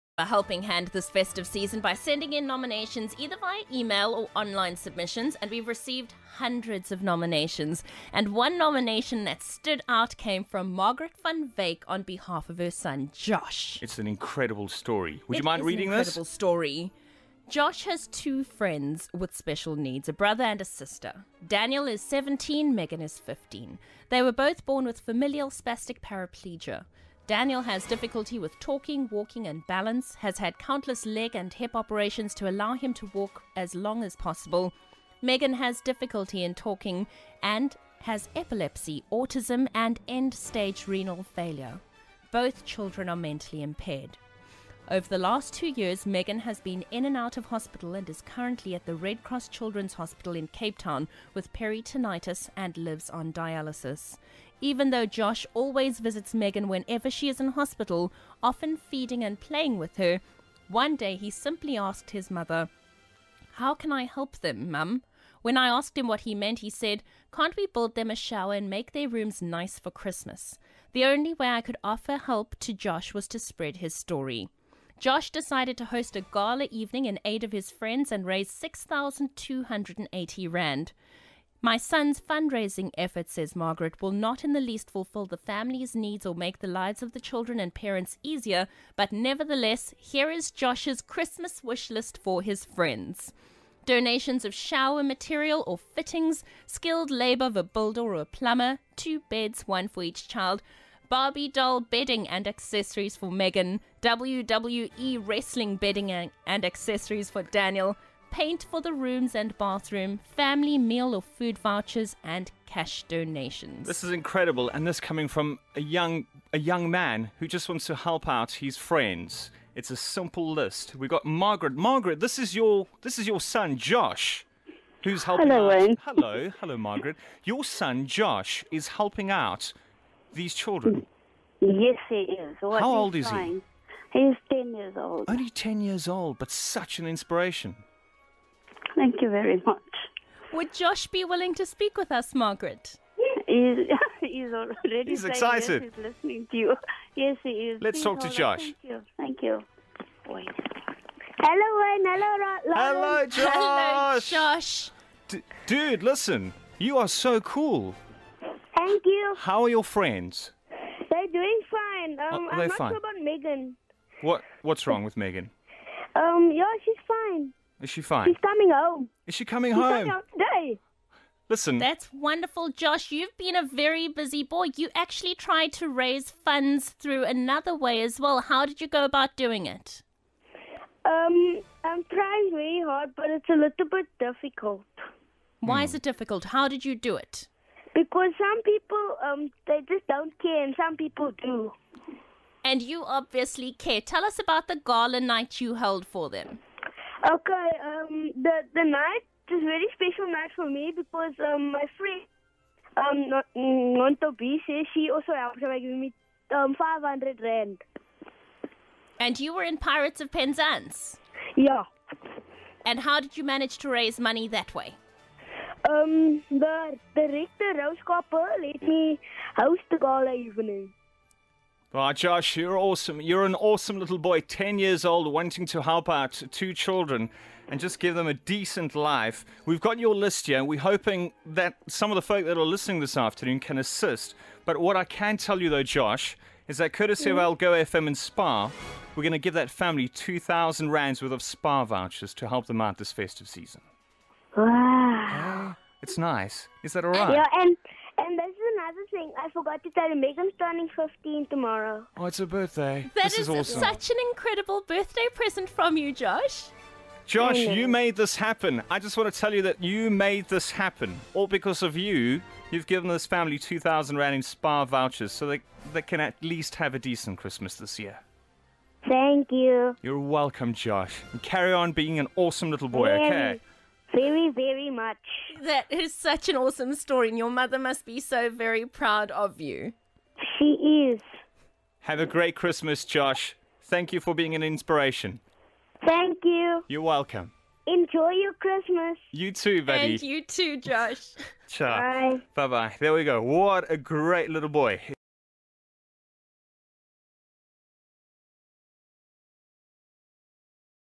9 year old Josh van Wyk builds a shower for sick siblings
Josh van Wyk raises funds to build a shower for siblings who are mentally and physically challenged, Megan in Renal Failure and Daniel needs another operation on legs